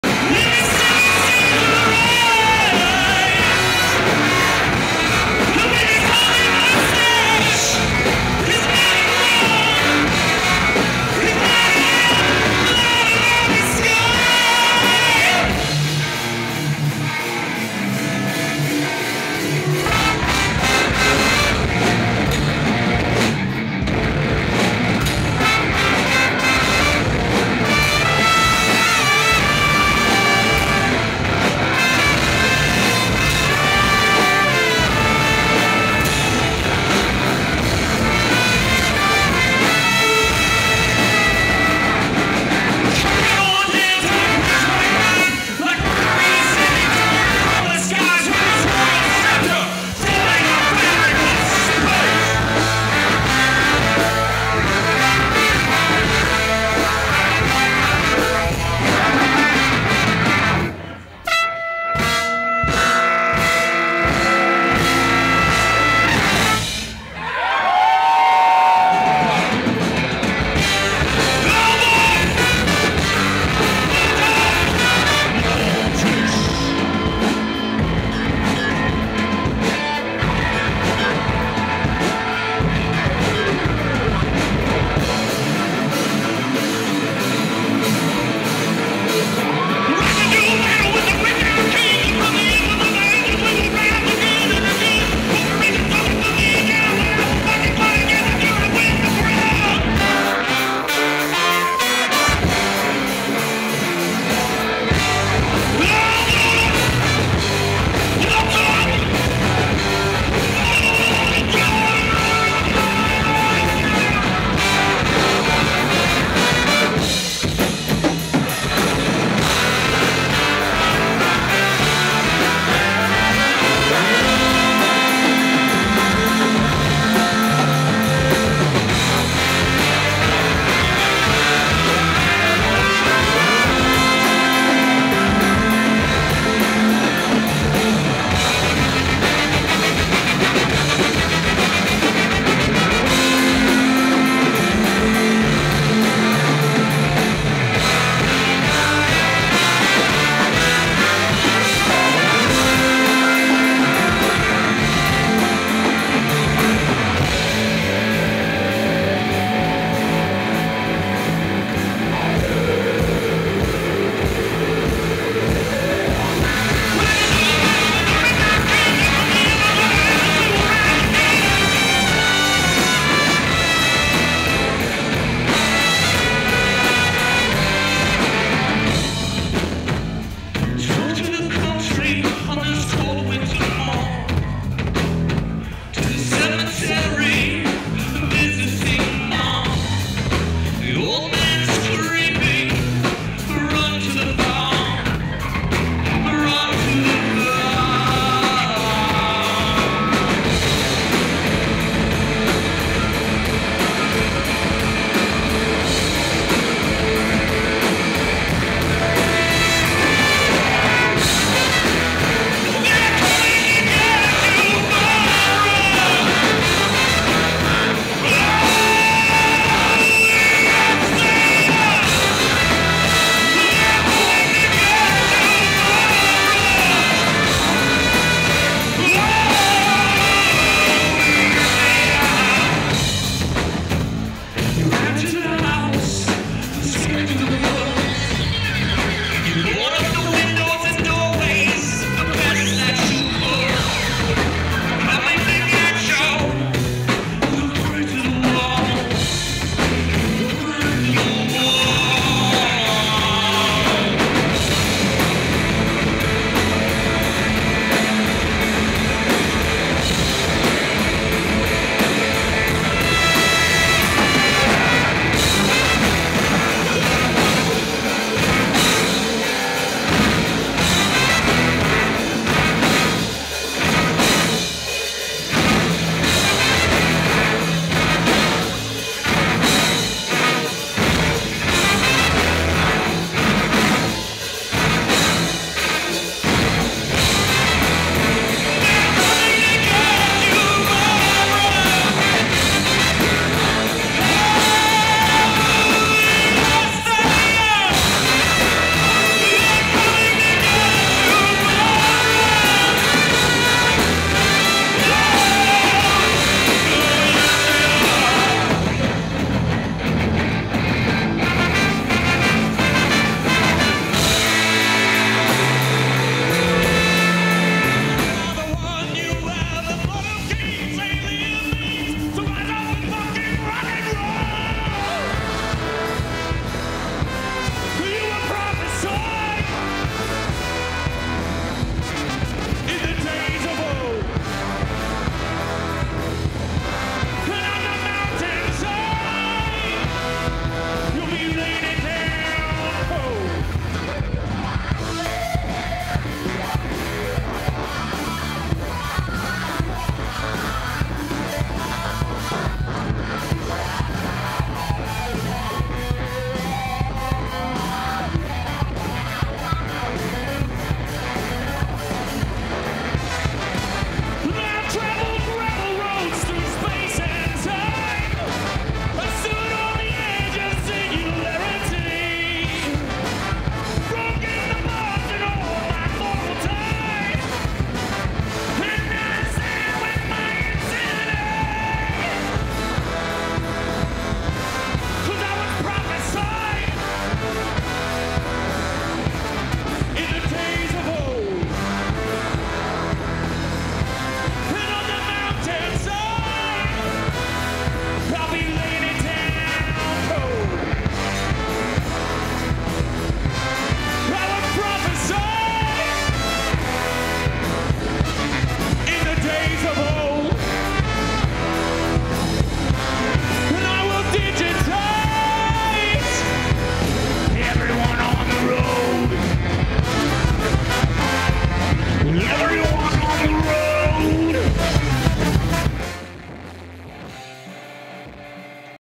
POTFL LIVE
Live video of Phoenix On The Faultline @ Locals only shot by Indypendent Productions